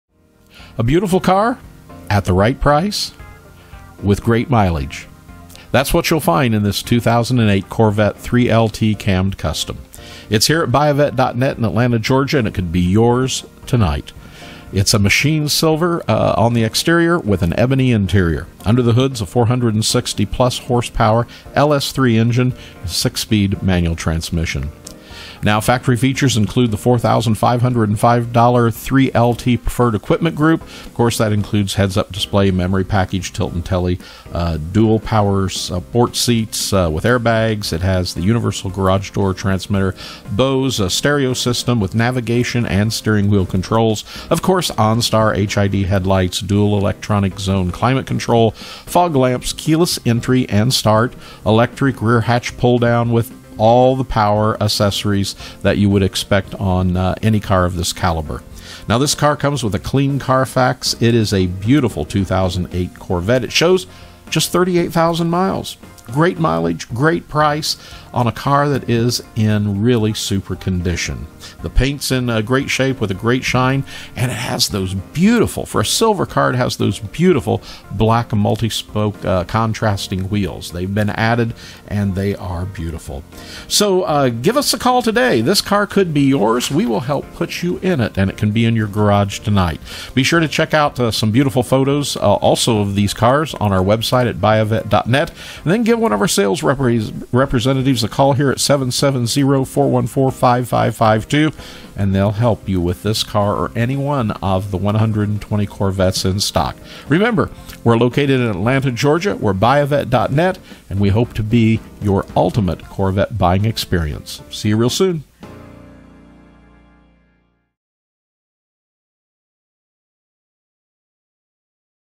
2008 Corvette 3LT Cammed Custom
2008 Corvette 3LT Cammed Custom For Sale

Machine Silver exterior, Ebony interior.

460+hp LS3 engine, 6 speed manual transmission, posi rear-end.

Factory features include the $4,505 3LT Preferred Equipment Group (Heads Up Display, memory package, tilt-n-tele, dual power heated sport seats with airbags, universal garage door transmitter), Bose CD/XM/MP3 stereo with Navigation and steering wheel controls, OnStar, HID headlights, dual zone electronic climate control, fog lamps, keyless entry and start, electric rear hatch pull-down, power windows, power locks, power steering, power brakes, power mirrors, cruise control, ABS, traction control, Active Handling.

Coming with a clean Carfax and LOADED with options, this 2008 Corvette shows 38,175 miles, and is in good condition. This car's paint is vg, with a great shine. A set of Black Niche multispoke wheels has been added, and Nitto NT555 tires show an average of 7/32nds tread depth remaining in the front, and an average of 8/32nds remaining in the rear. The interior of this 2008 Corvette shows vg factory leather seats and door panels, and good-vg steering wheel and center console. Other additions to this car include a mild aftermarket performance camshaft, RPM rear differential, window tint, blacked-out side markers, short throw shifter, Halltech cold air intake, MSD spark plug wires, and a speed bleeder for the clutch master cylinder. Alternator appears to be newer, and the subframe is corrosion-free. 38K Miles.